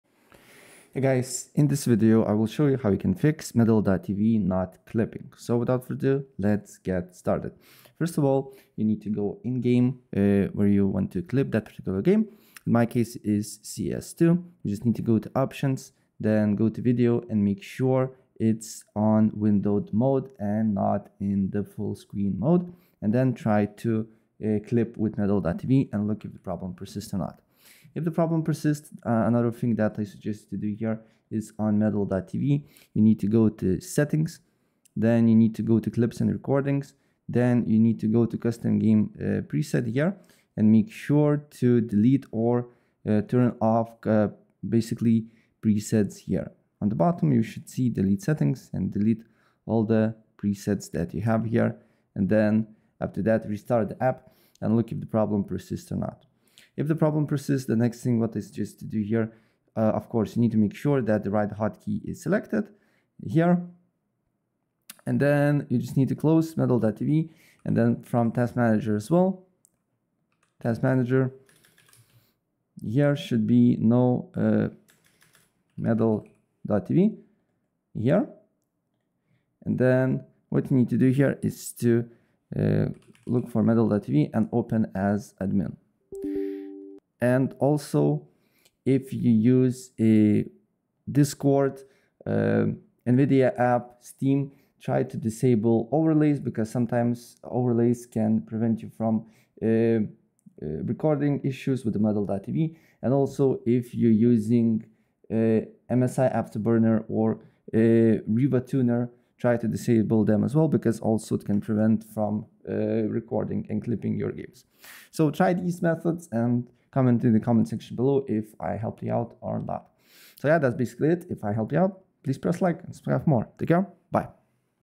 How To Fix Medal.tv Not Clipping (Updated 2025)
In this video I will show you How To Fix Medal.tv Not Clipping. It's really easy and it will take you less than a minute to do it!